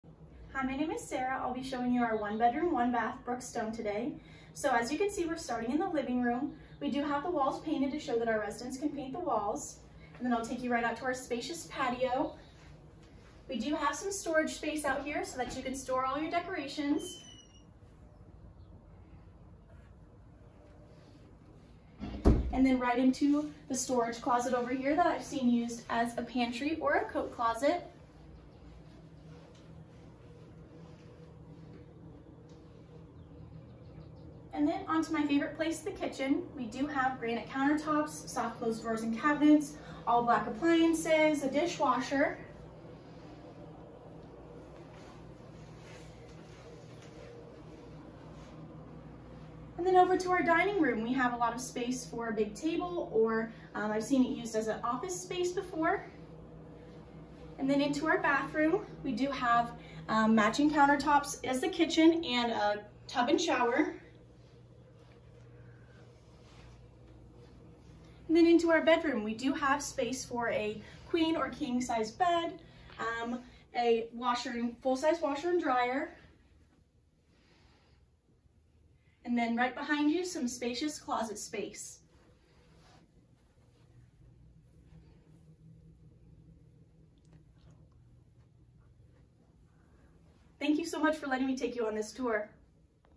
Brookstone 1x1: 720 square feet (COMPLETE RENOVATED)
The Brookstone home is a Reserve at Barry favorite. At 720 square feet, the one bed one bath home offers all electric black appliances, a full sized washer and dryer already installed, two oversize closets and spacious storage off the patio or balcony.